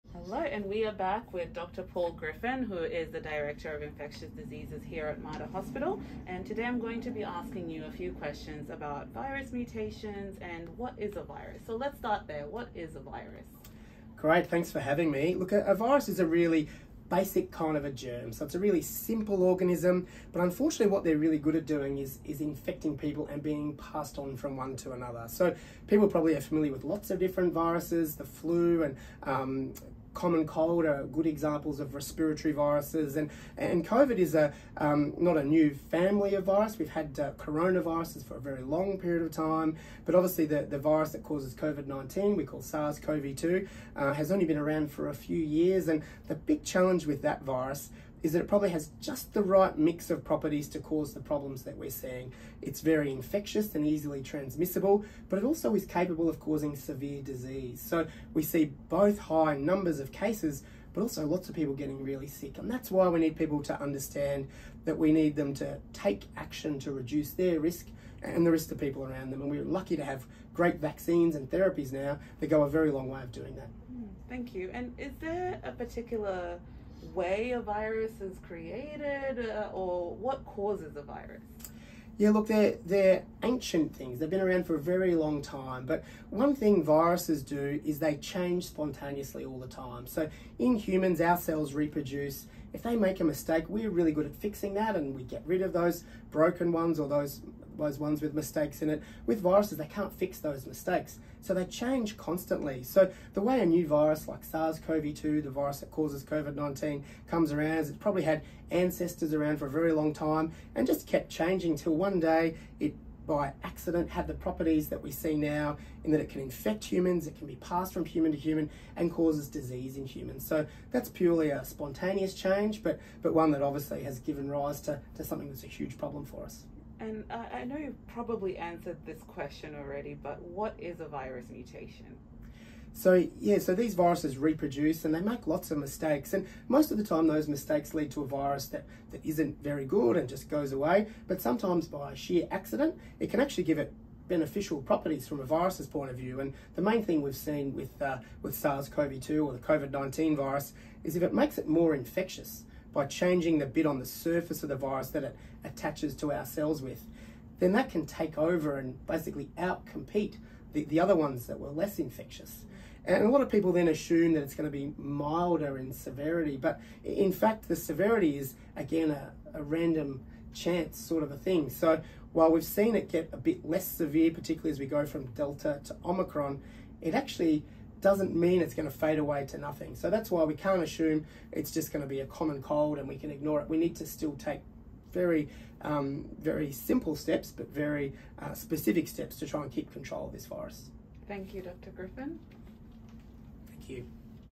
Dr Griffin on Viruses & Mutations
CCHEP Partnership and Community Engagement Coordinator, Grace in conversation with Dr Paul Griffin, Director of Infectious Diseases at Mater Hospital. Finding out more about viruses and how they mutate.